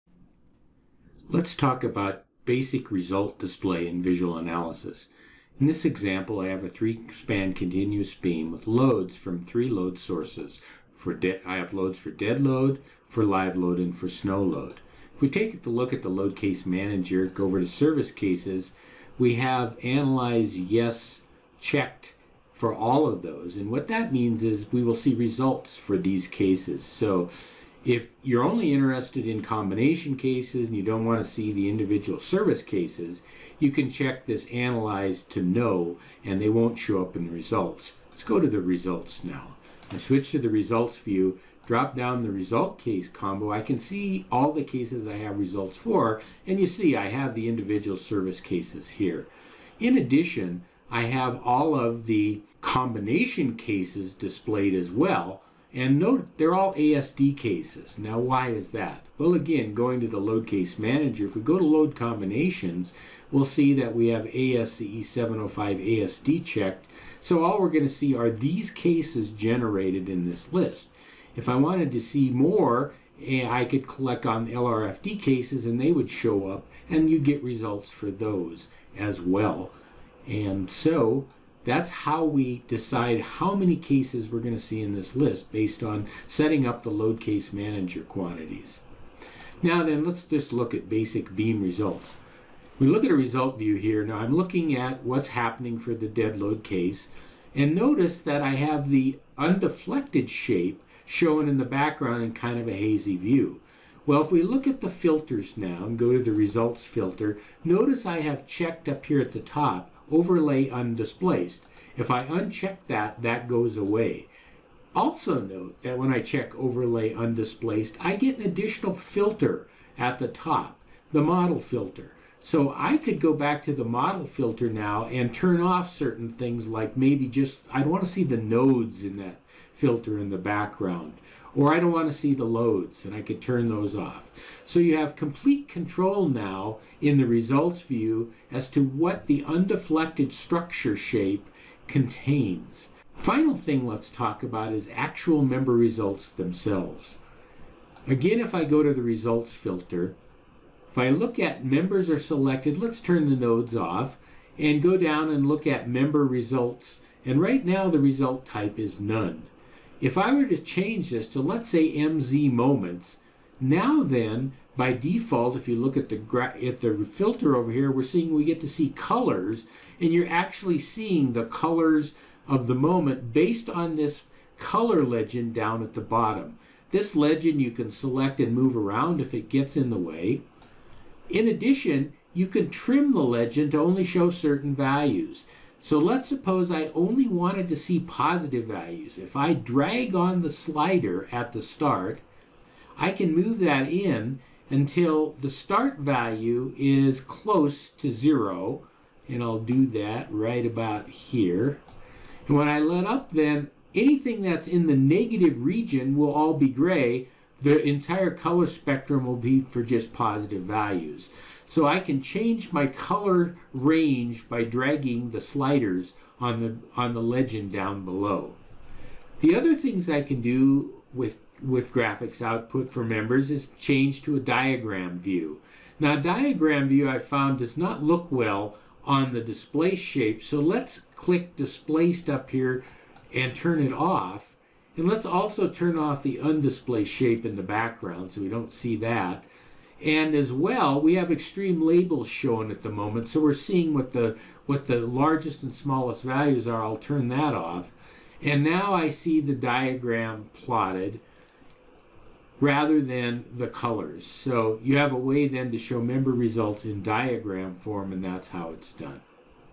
VisualAnaysis: Basic Analysis Results
Basic analysis results and filtering in the result view is discussed. Member results display is demonstrated.